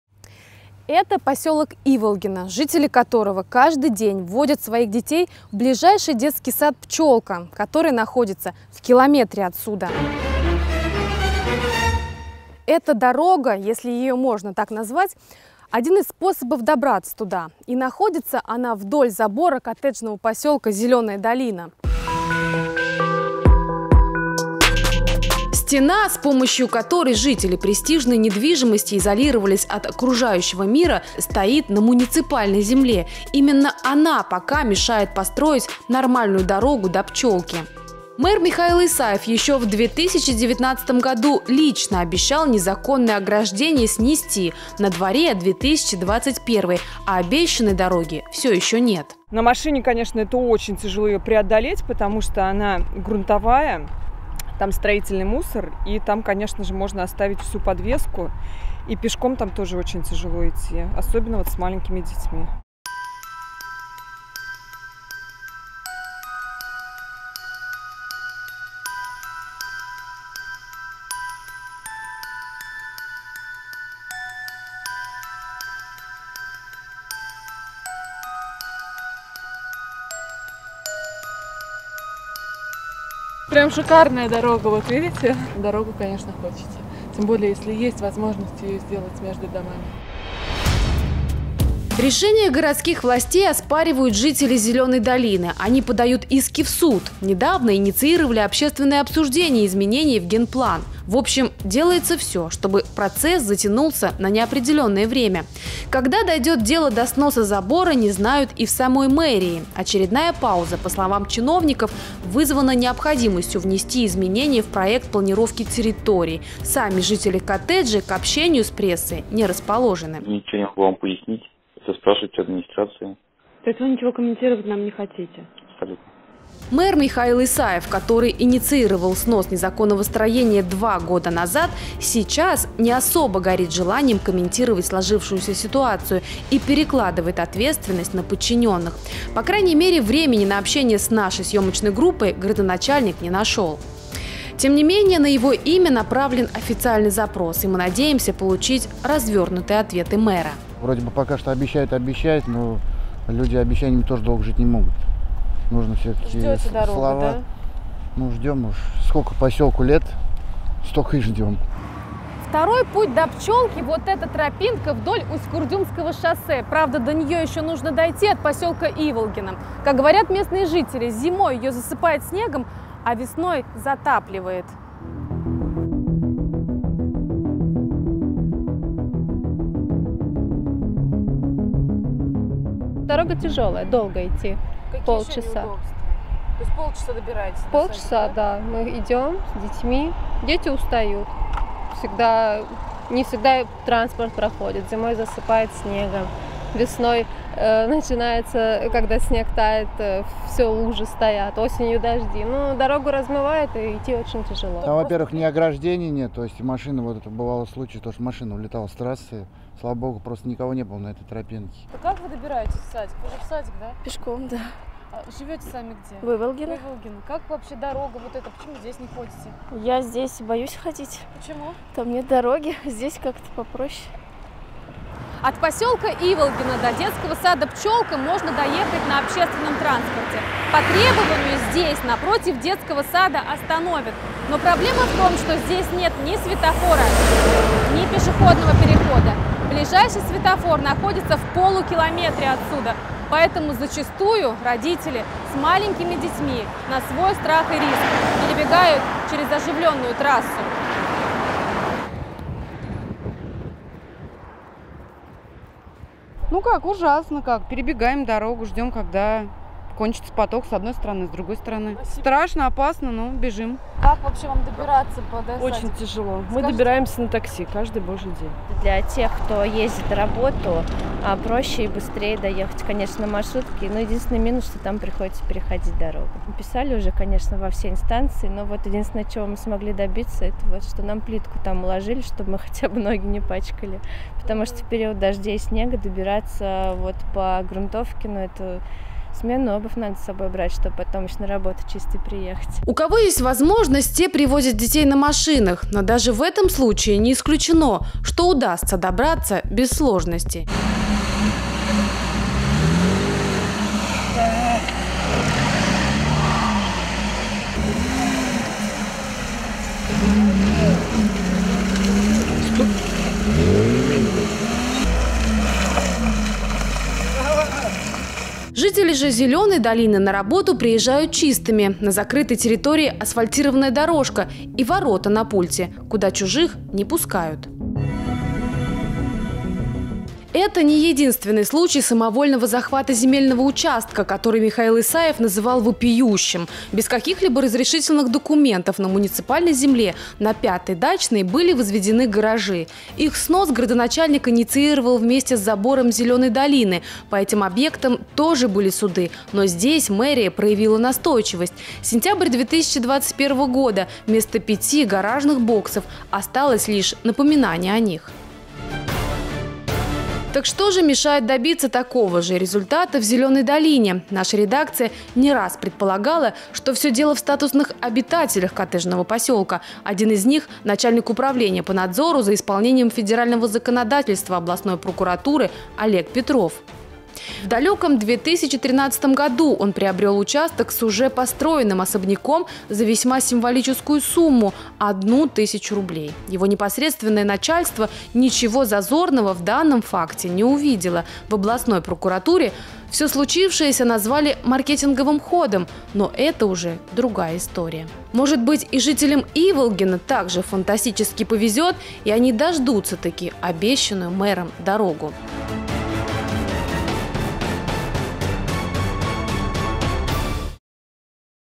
Забор прокурорской Рублевки в Саратове
Снос забора коттеджного поселка "Зеленая долина" в Саратове вновь отложен на неопределенное время. Ограждение "прокурорской Рублевки" мешает строительству дороги от многоэтажных домов микрорайона Иволгино к детскому саду "Пчелка".